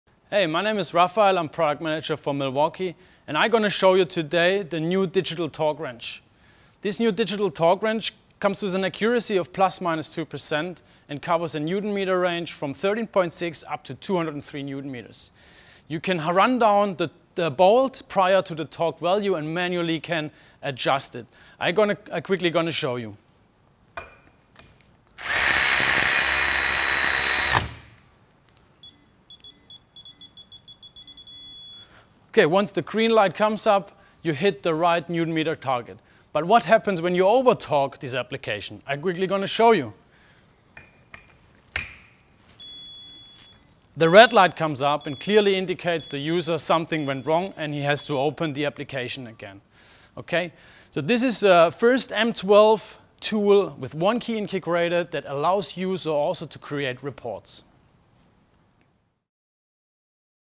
Milwaukee® M12 FUEL™ Digital Torque Wrench - Product Manager Demo
Our Milwaukee product manager talks you through our new M12 FUEL™ Digital Torque Wrench. This new digital torque wrench comes with an accuracy of +/- 2% and cover a range of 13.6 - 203Nm. You can tighten the bolt with the ratchet and then use the tool to manually get the correct torque and once the green light comes up you have hot the correct Nm target. This is turning what is currently a two tool application into a single tool application.

The tool has ONE-KEY™ integrated allowing users to create reports.